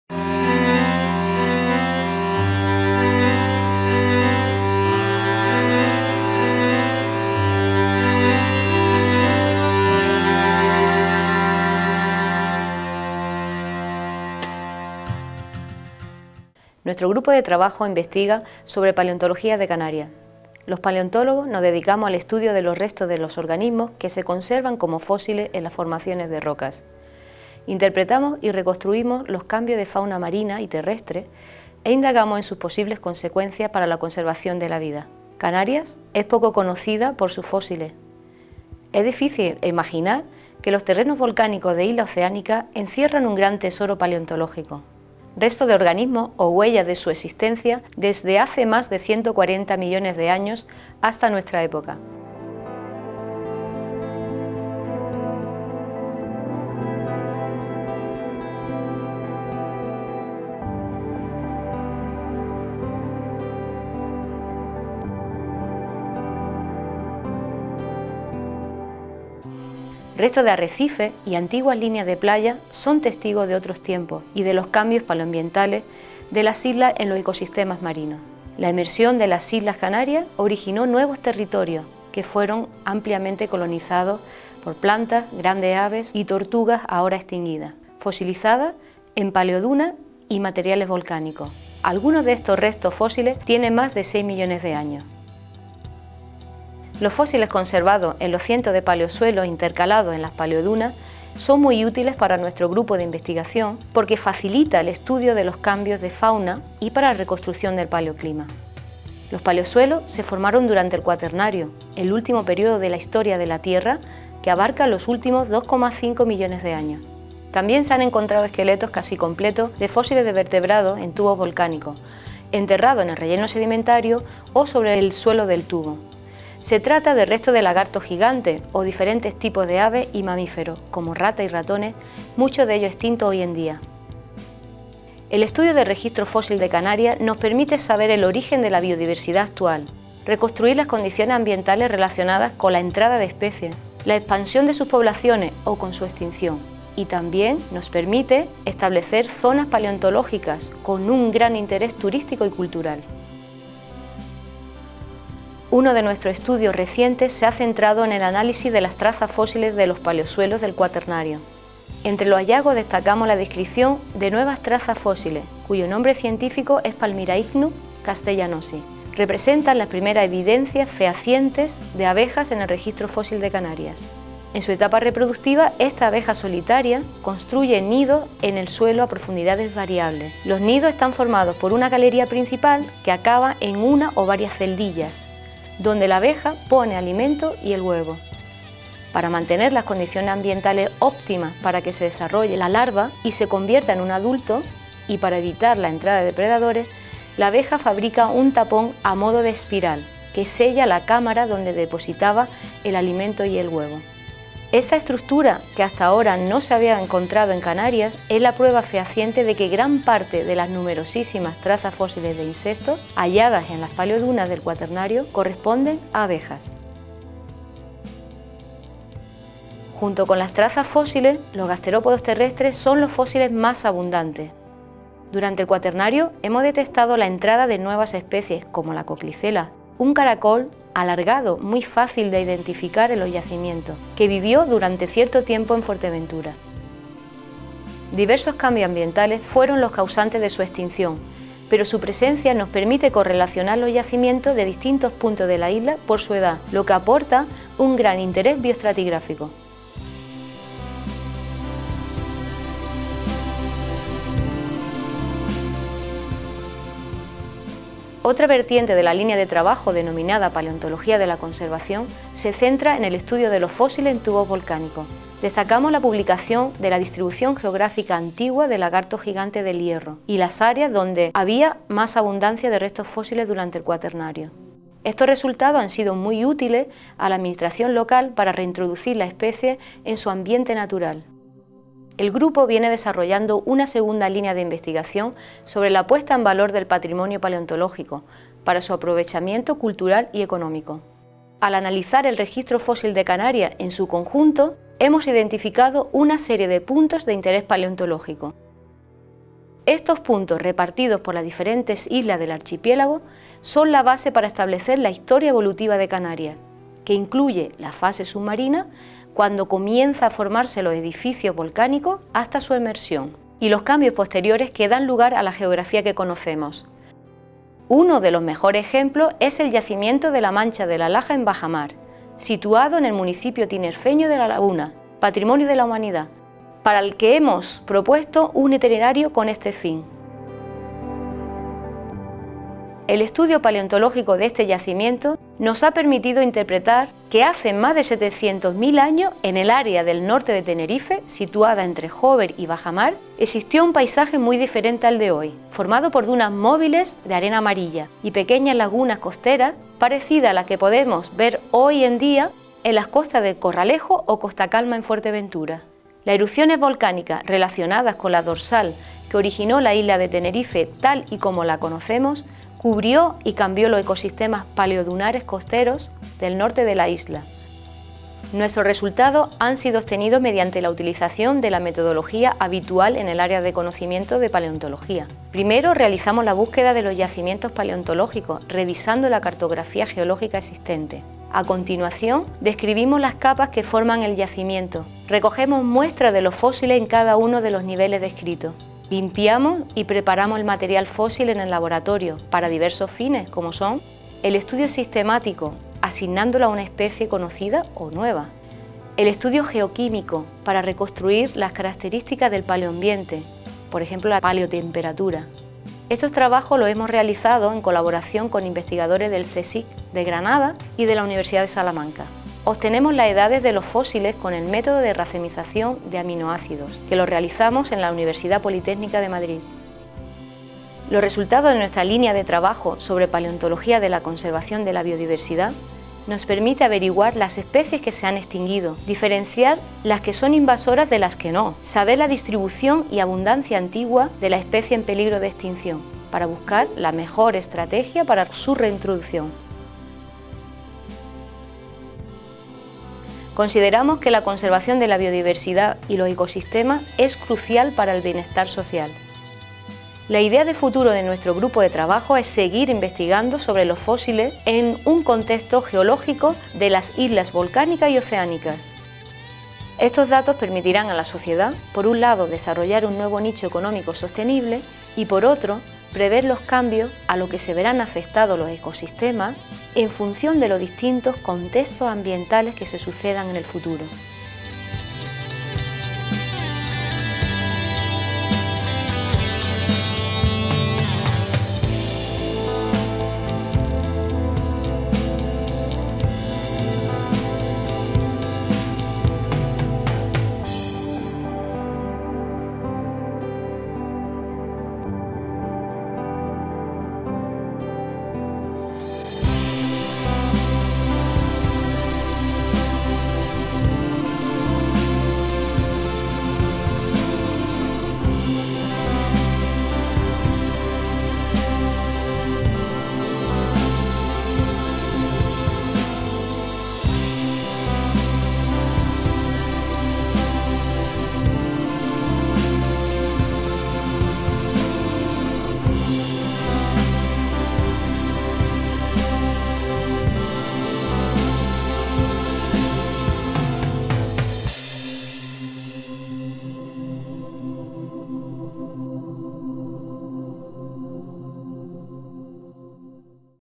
ULLresearch: El registro fósil de Canarias. Más de 140 millones de años de Historia Natural
Este vídeo es una muestra del amplio y variado Registro Fósil de las islas Canarias, conservado en un contexto geológico de islas volcánicas y oceánicas. Incluye sedimentos de fondos oceánicos del Mesozoico con fósiles de ammonoideos, restos de antiguas líneas de playas del Neógeno y Cuaternario, fósiles de gasterópodos terrestres y trazas fósiles de abejas y otros insectos conservados en paleosuelos y paleodunas, y restos de vertebrados en tubos volcánicos principalmente.
Destacamos la utilidad del estudio del Registro Fósil de Canarias en dos líneas de investigación desarrolladas en el Área de Paleontología de la Universidad de La Laguna: Paleontología de la Conservación de la Biodiversidad que nos permite conocer las especies que se han extinguido, diferenciar las que son invasoras de las endémicas, saber la distribución y abundancia antiguas de especies en peligro de extinción para ayudar a establecer una mejor estrategia de reintroducción. Y, la puesta en valor científico, sociocultural y socioeconómico de yacimientos paleontológicos para ofrecer nuevos productos culturales y turísticos. En Canarias donde el sector económico más importante es el Turismo, los resultados de estos estudios contribuirán a dinamizar y diversificar dicho sector, y por ende el desarrollo económico de las islas.